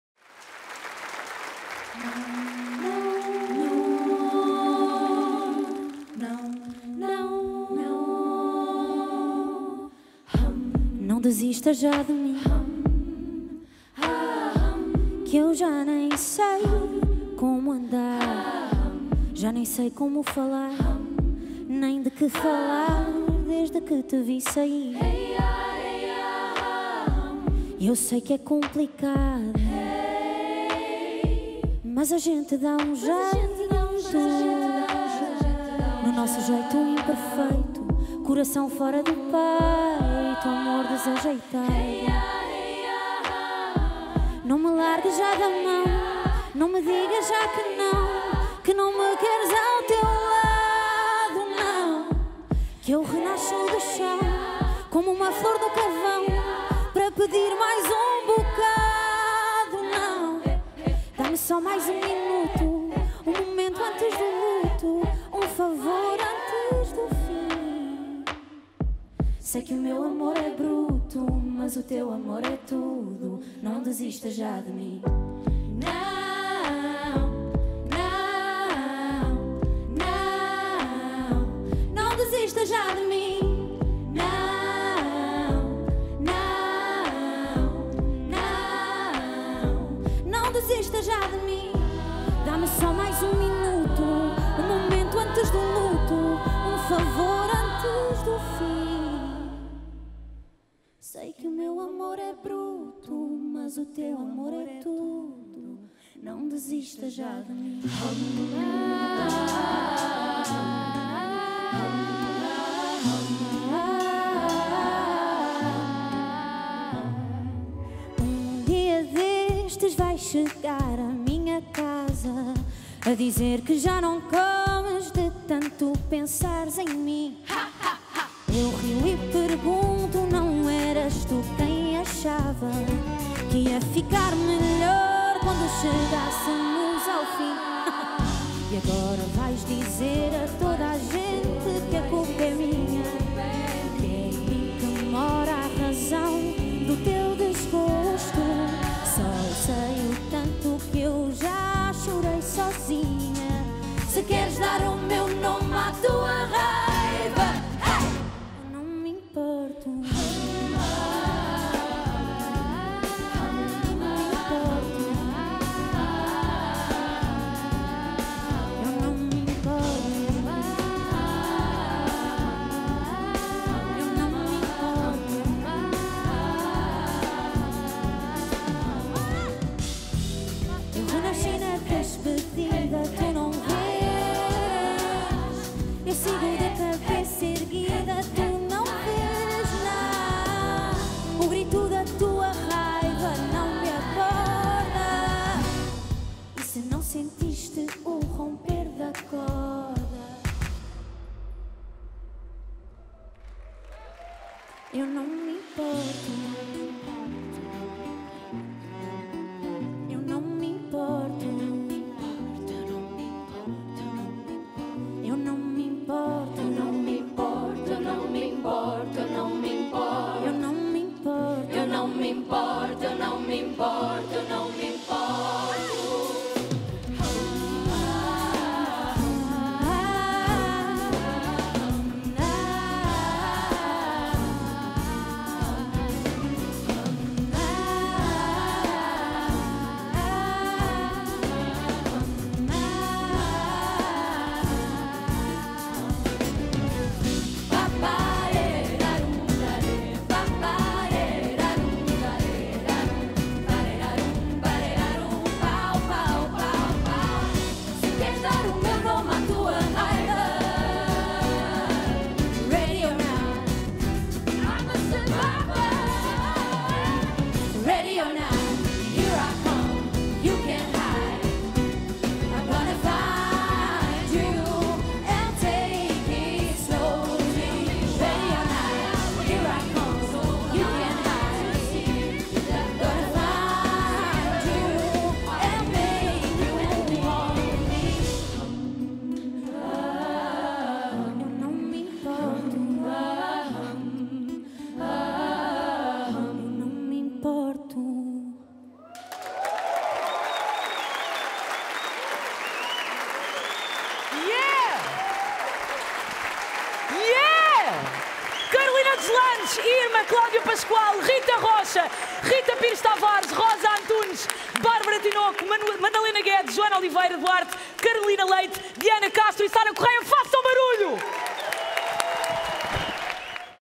Carolina Deslandes - Actuação Prémios Play
Carolina Deslandes numa actuação inédita ao lado de 11 vozes femininas: Bárbara Tinoco, Carolina Leite, Cláudia Pascoal, Diana Castro (que é também autora dos arranjos vocais), Inês Pires, IRMA, Joana Duarte, Madalena Guedes, Rita Rocha, Rosa Antunes e Sara Correia, acompanhadas pela banda composta por André Silva, Feodor Bívol, Marco Pombinho, Ricardo Marques e Sandra Martins.